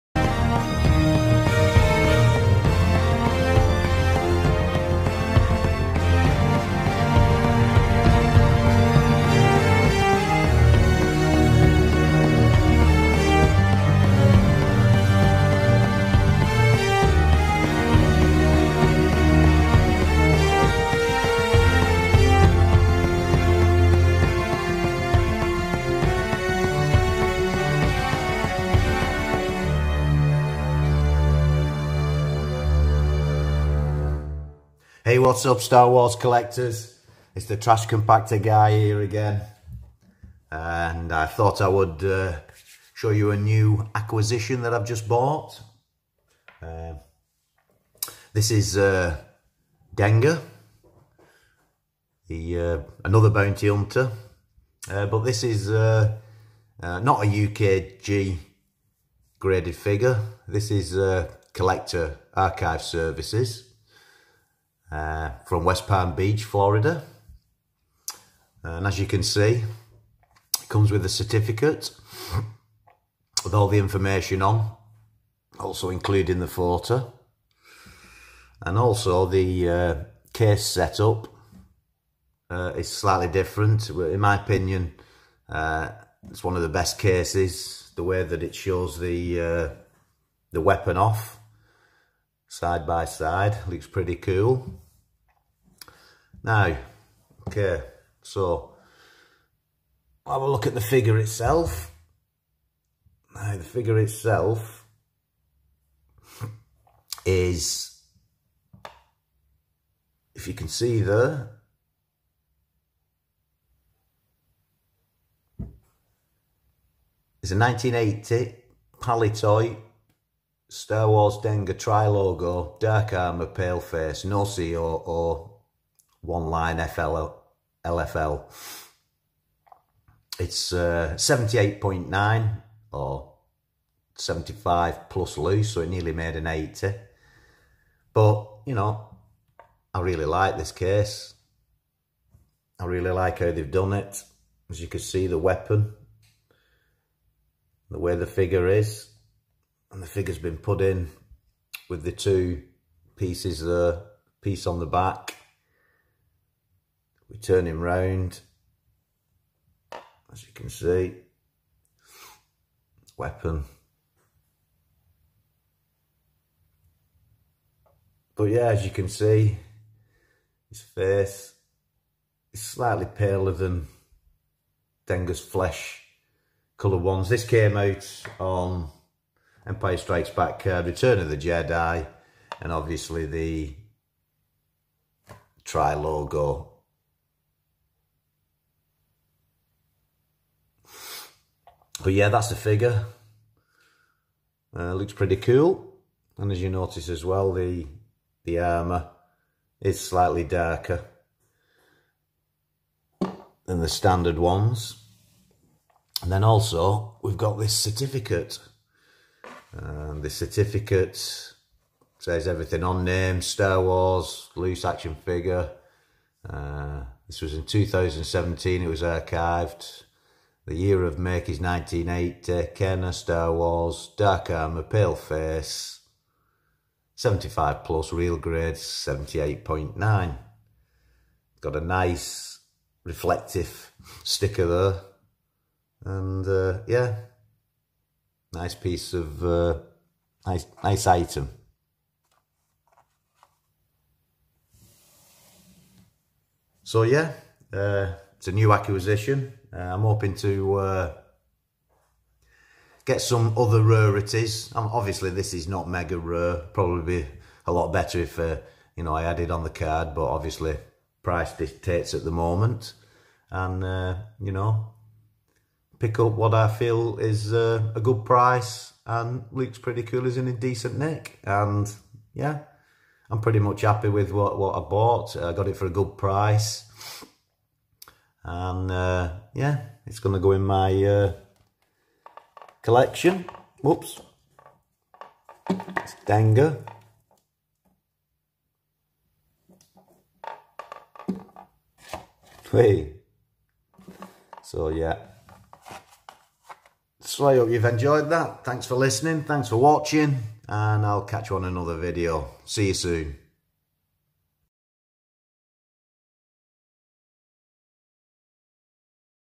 Dengar Tri Logo/PBP scar CAS Graded star wars Figure.
Just added this CAS Graded palitoy tri logo/PBP scar Dengar star wars figure to my collection.